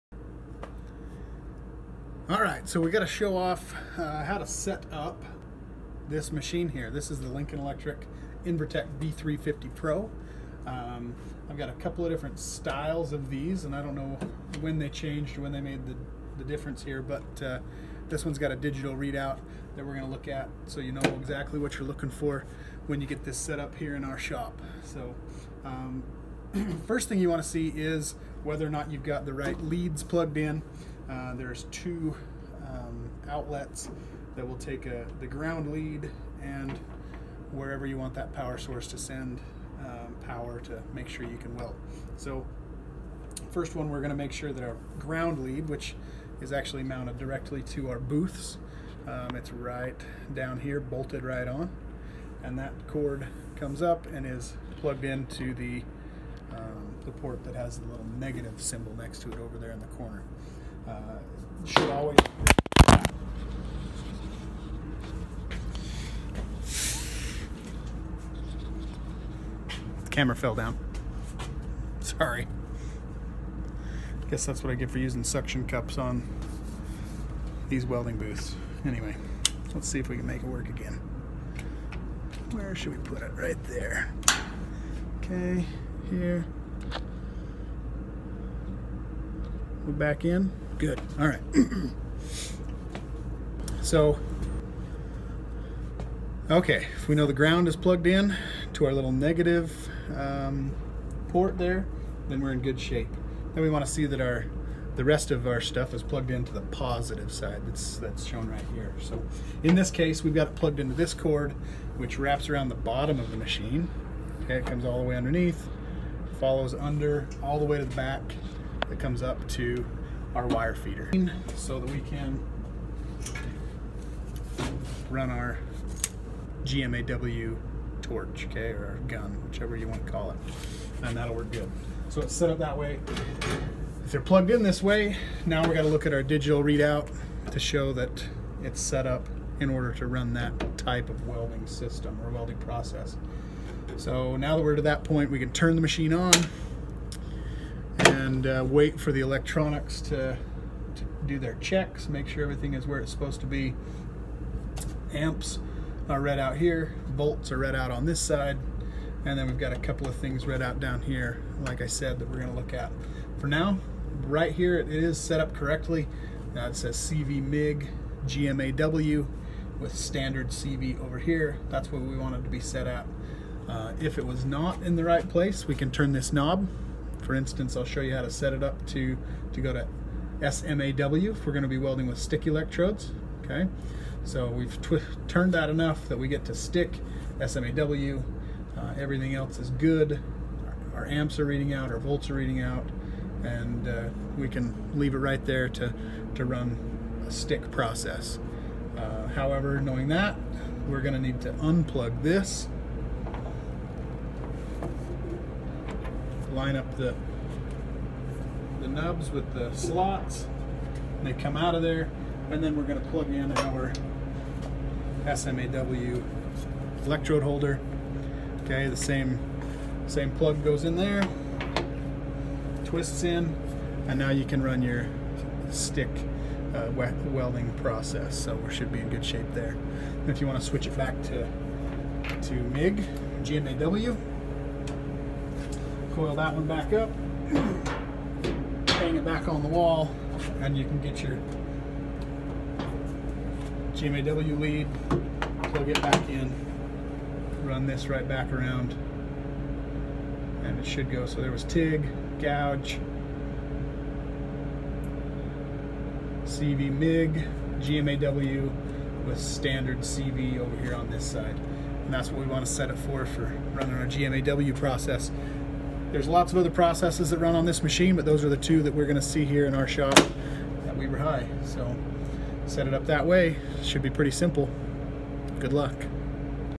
Setting up the V350 Pro - One Day At A Time with Dave from AgTeachering
Video for my students so they can easily set up the machine without having to track me down.